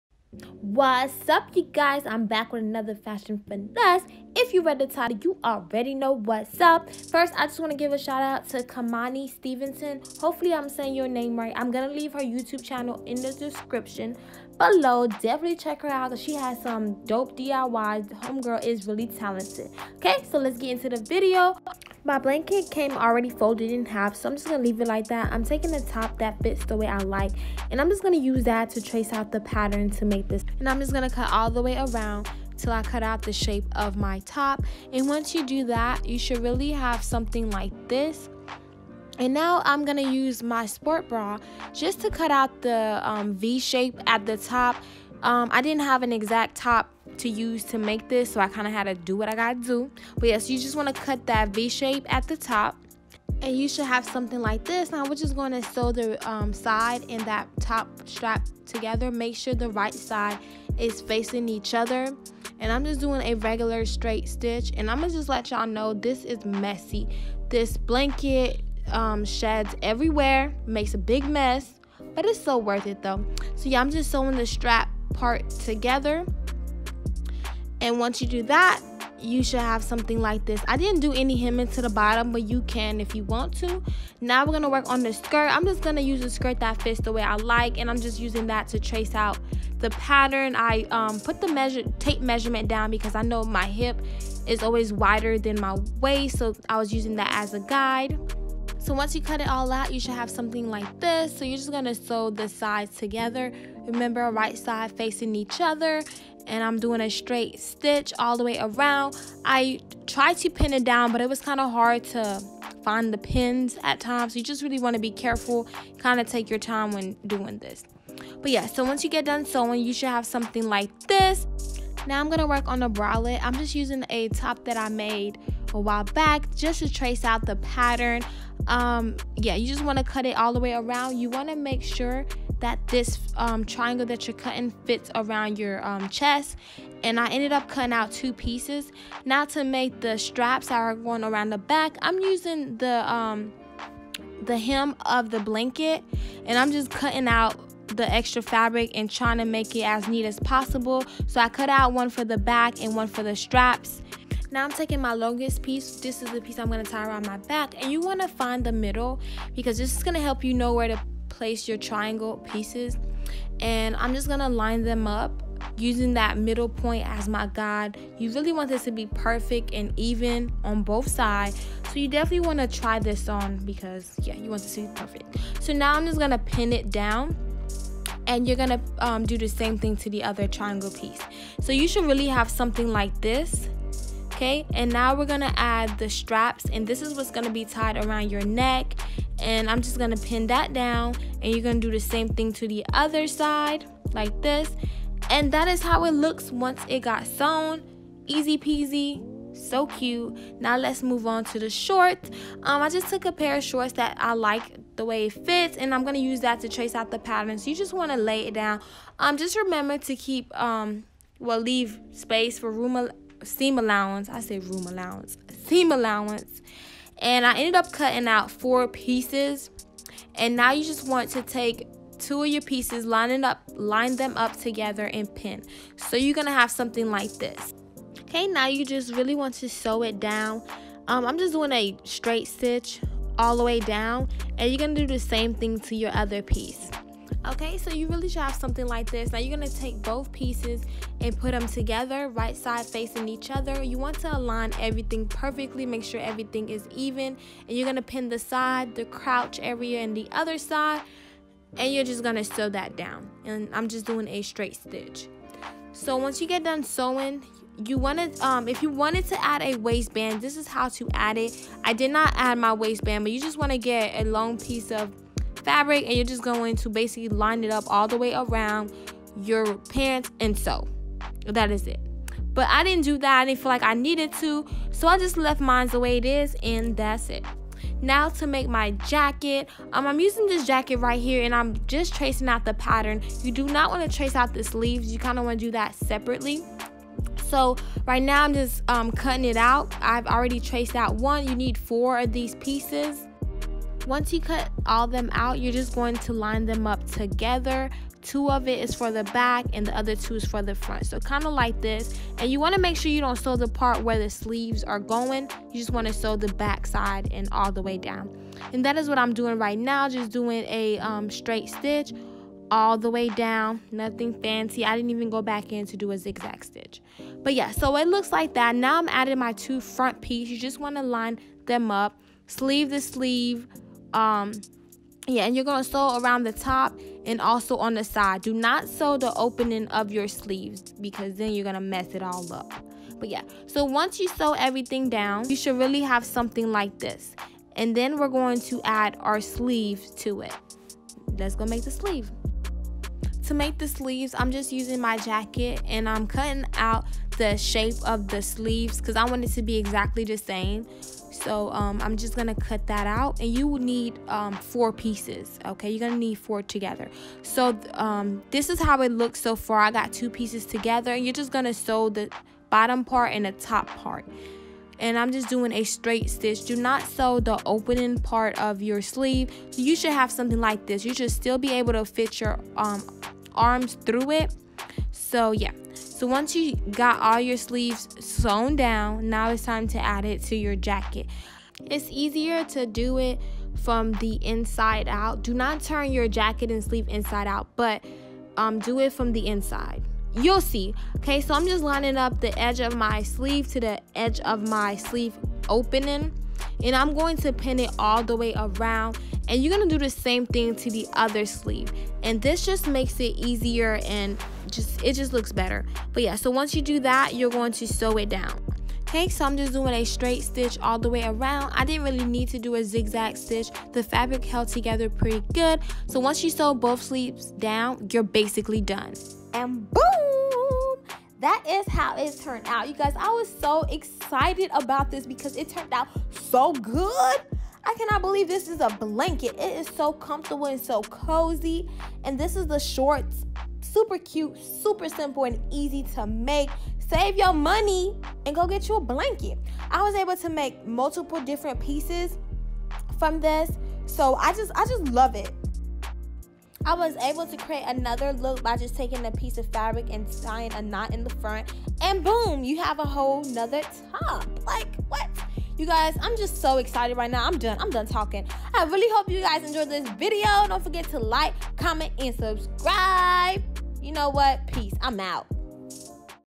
DIY $25 3 piece Fuzzy Set | Fashion Finesse | ChrisCentric | HOW TO
Hey guys!! I’m back with another Fashion Finesse. I finally tried making my own fuzzy set from a $25 blanket from Walmart. This video was inspired by Khuemani Stevenson.  Y’all should definitely check her out! I hope you guys enjoy this video!!